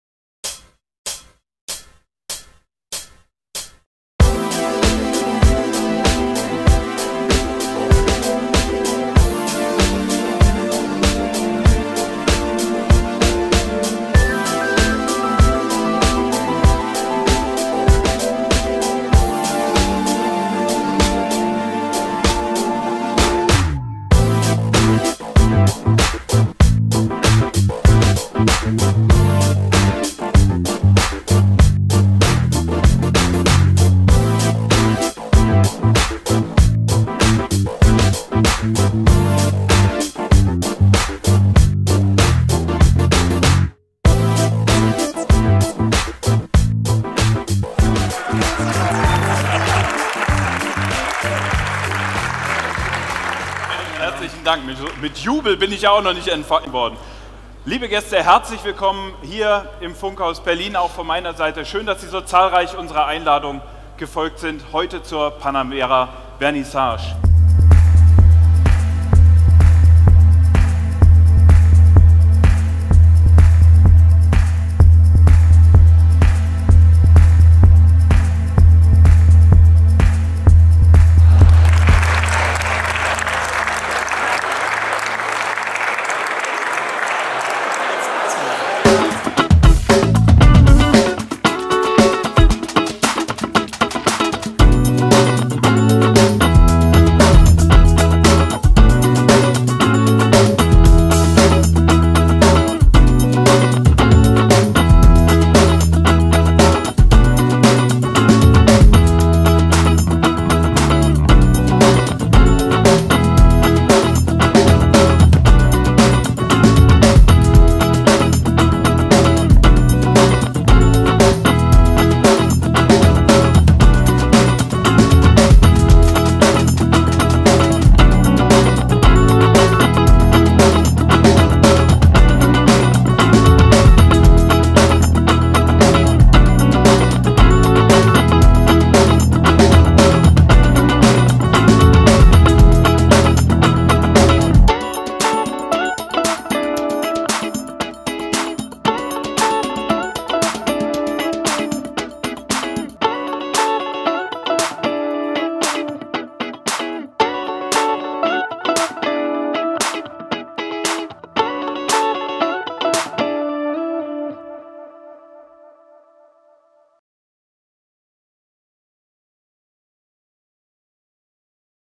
Eventvideo zur Premiere des neuen Porsche Panamera 2016 in Berlin
Exklusive Veranstaltung der Porsche Zentren in Berlin zur Vorstellung des neuen Panamera.